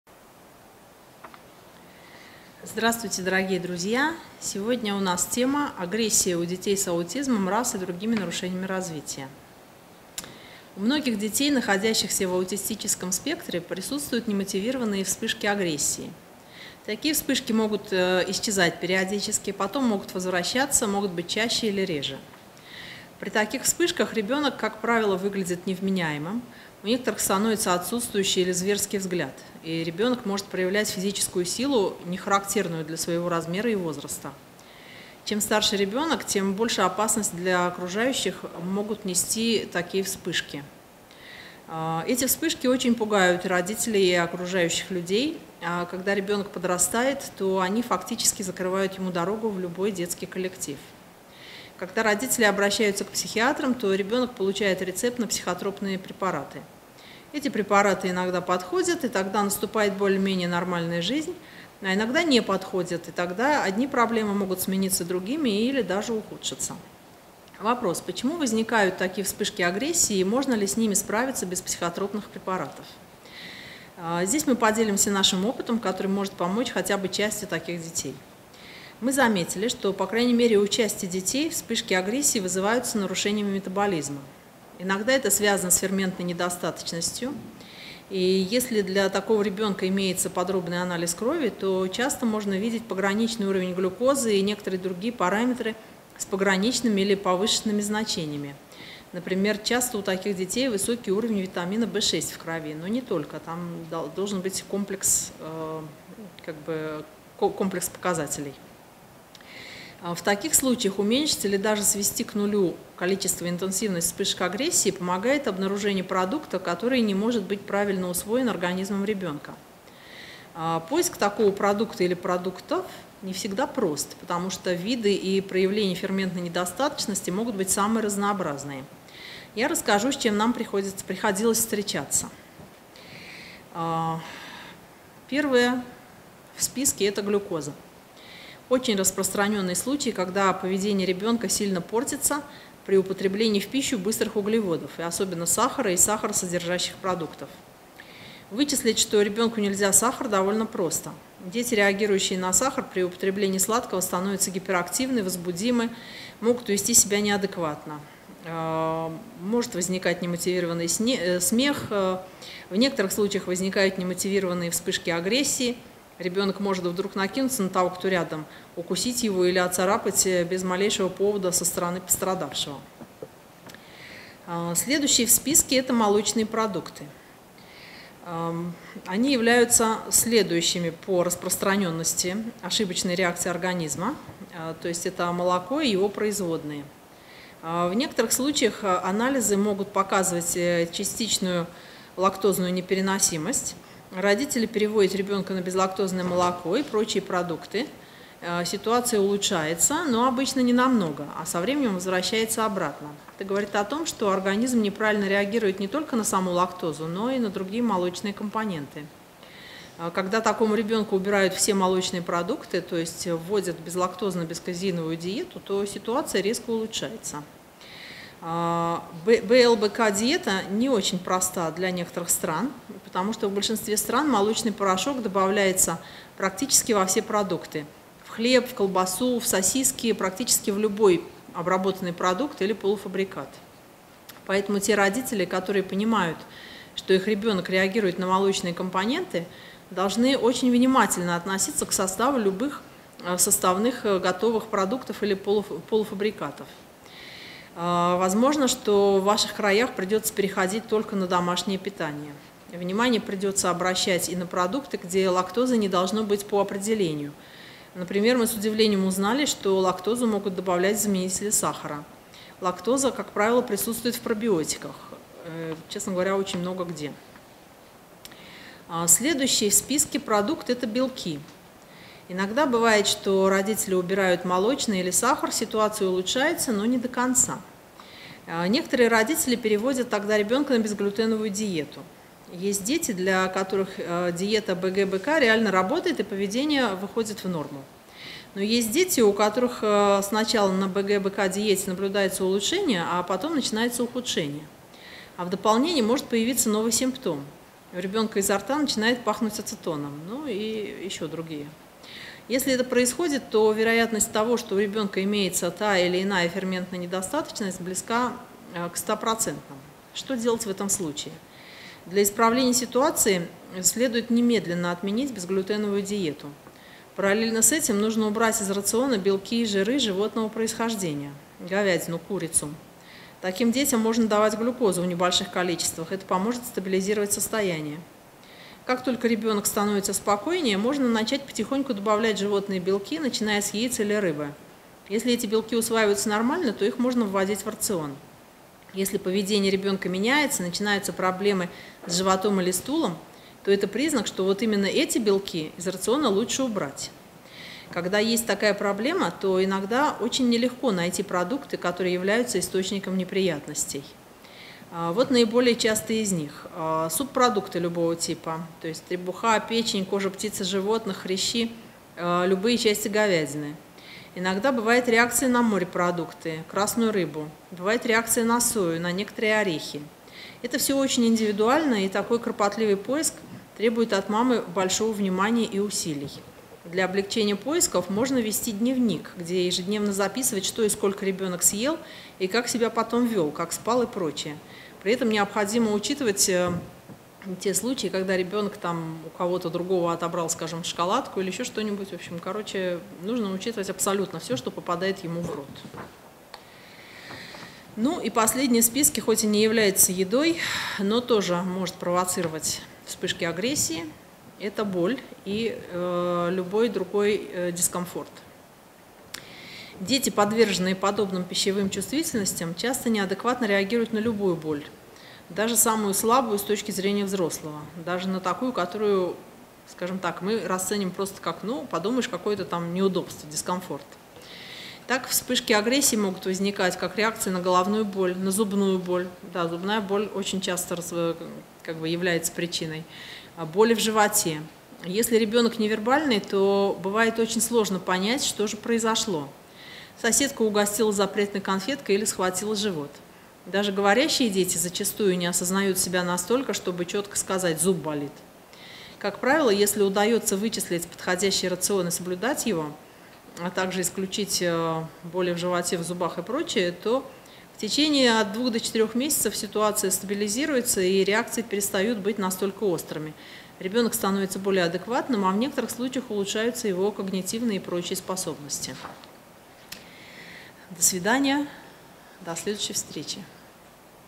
Агрессия у детей с аутизмом, РАС и другими нарушениями развития. Центр BEER DAVID
У многих детей, находящихся в аутистическом спектре, присутствуют немотивированные вспышки агрессии.
При таких вспышках ребенок как правило выглядит невменяемым, у некоторых становится отсутствующий или зверский взгляд, и ребенок может проявлять физическую силу, нехарактерную для своего размера и возраста. Чем старше ребенок, тем больше опасности для окружающих могут нести такие вспышки.

Когда родители обращаются к психиатрам, то ребенок получает рецепт на психотропные препараты. Эти препараты иногда подходят, и тогда наступает более или менее нормальная жизнь, а иногда не подходят, и тогда одни проблемы могут смениться другими или даже ухудшиться.
Вопрос: почему возникают такие вспышки агрессии и можно ли с ними справиться без психотропных препаратов?
Мы заметили, что, по крайней мере, у части детей вспышки агрессии вызываются нарушениями метаболизма. Иногда это связано с ферментной недостаточностью.
Если для такого ребенка имеется подробный анализ крови, то часто можно видеть пограничный уровень глюкозы и некоторые другие параметры с пограничными или повышенными значениями. Например, часто у таких детей высокий уровень витамина Б6 в крови.
В таких случаях уменьшить или даже свести к нулю количество и интенсивность вспышек агрессии помогает обнаружение продукта, который не может быть правильно усвоен организмом ребенка.
Поиск такого продукта или продуктов не всегда прост, потому что виды и проявления ферментной недостаточности могут быть самые разнообразные.
Первая в списке – глюкоза.
Вычислить, что ребенку нельзя сахар, довольно просто. Дети, реагирующие на сахар, при употреблении сладкого становятся гиперактивны, возбудимы, могут вести себя неадекватно.
Молочные продукты – следующие в списке.
В некоторых случаях анализы могут показывать частичную лактозную непереносимость, родители переводят ребенка на безлактозное молоко и прочие продукты, ситуация улучшается, но ненамного, а со временем возвращается обратно. Это говорит о том, что организм неправильно реагирует не только на саму лактозу, но и на другие молочные компоненты.
Когда такому ребенку убирают все молочные продукты, то есть вводят безлактозно-безказеиновую диету, ситуация резко улучшается.
БЛБК диета не очень проста для некоторых стран, потому что в большинстве стран молочный порошок добавляется практически во все продукты: в хлеб, колбасу, сосиски, практически в любой обработанный продукт или полуфабрикат.
Белки.Иногда бывает, что родители убирают молочное или сахар, ситуация улучшается, но не до конца.Есть дети, у которых сначала на БГБК диете наблюдается улучшение, а потом начинается ухудшение. А в дополнение может появиться новый симптом: у ребенка изо рта начинает пахнуть ацетоном.
Если это происходит, то вероятность того, что у ребенка имеется та или иная ферментная недостаточность, близка к 100 %.
Для исправления ситуации следует немедленно отменить безглютеновую диету. Параллельно с этим нужно убрать из рациона белки и жиры животного происхождения (говядину, курицу).
Как только ребенок становится спокойнее, можно начать потихоньку добавлять животные белки, начиная с яиц или рыбы. Если эти белки усваиваются нормально, то их можно вводить в рацион.
Если поведение ребенка меняется, начинаются проблемы с животом или стулом, то это признак, что вот именно эти белки из рациона лучше убрать.
Вот наиболее частые из них: субпродукты любого типа (требуха, печень, кожа птиц и животных, хрящи), любые части говядины. Иногда бывает реакция на морепродукты, красную рыбу. Бывает реакция на сою, некоторые орехи.
Это все очень индивидуально, и такой кропотливый поиск требует от мамы большого внимания и усилий.
Для облегчения поисков можно вести дневник, где ежедневно записывать, что и сколько ребенок съел, и как себя потом вел, как спал и прочее. Необходимо учитывать случаи случайной пищи: кто-то угостил и т.д.
Боль и любой другой дискомфорт также могут провоцировать вспышки агрессии.
Дети, подверженные подобным пищевым чувствительностям, часто неадекватно реагируют на любую боль, даже самую слабую с точки зрения взрослого. Так, вспышки агрессии могут возникать, как реакция на головную боль, зубную боль, боль в животе.
Как правило, если удается вычислить подходящий рацион и соблюдать его, то в течение 2-4 месяцев ситуация стабилизируется и реакции перестают быть настолько острыми.

НОВОЕ!!! Энциклопедия практической помощи при аутизме и ДЦП